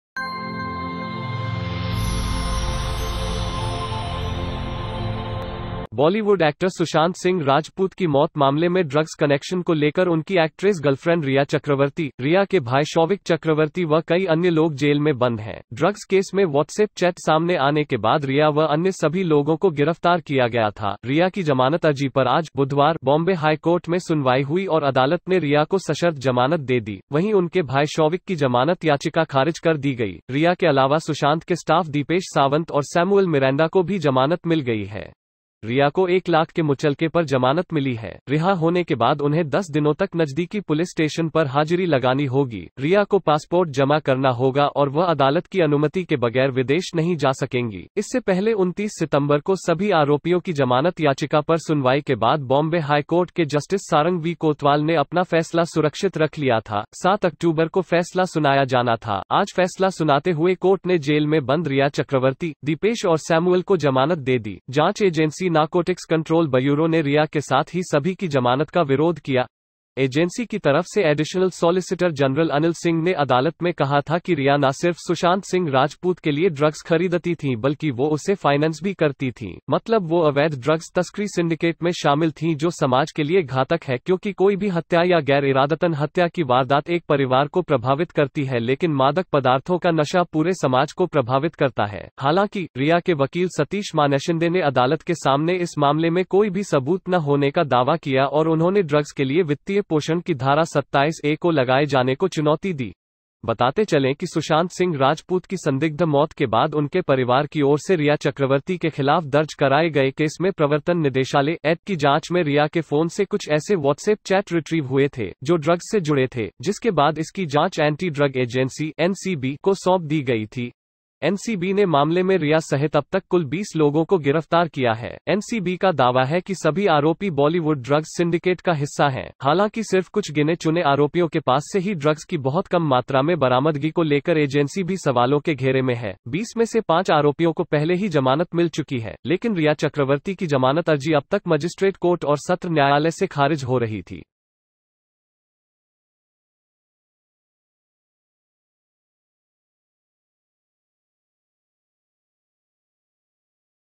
रिया चक्रवर्ती को मिल गयी बेल || Rhea Chakraborty get Bail from Bombay High Court || Rhea Bail news-
Troll / Like / Follow us on